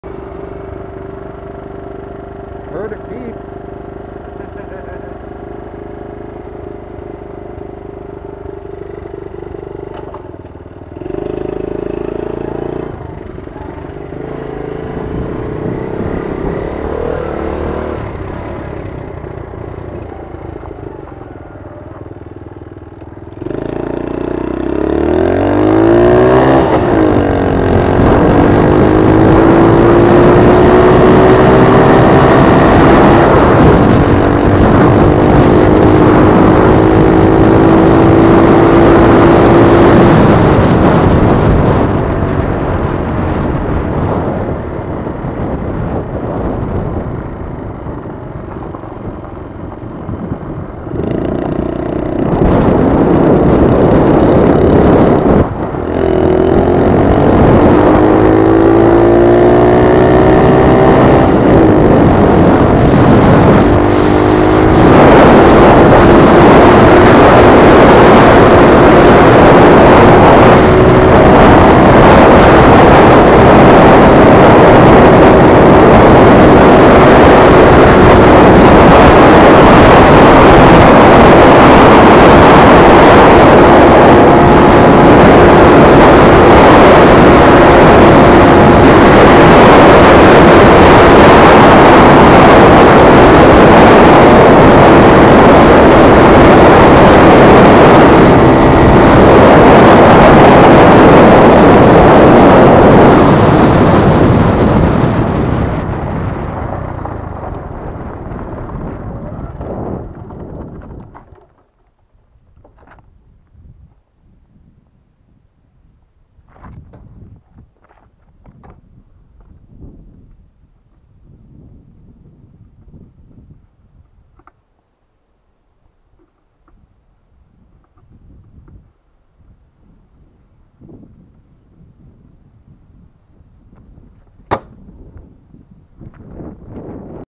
1970 Honda CT70 - dirt road
playing with the cam and the bike